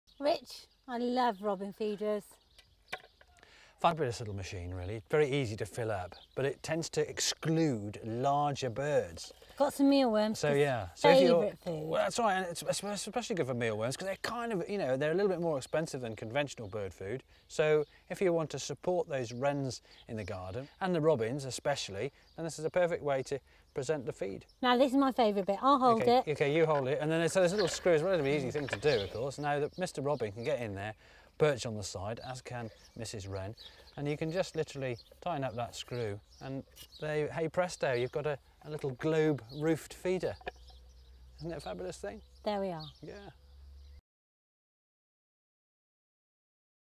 Robin Feeder
Heather and Richard fill a Robin Feeder with Mealworms, then lower the dome to keep the larger birds out.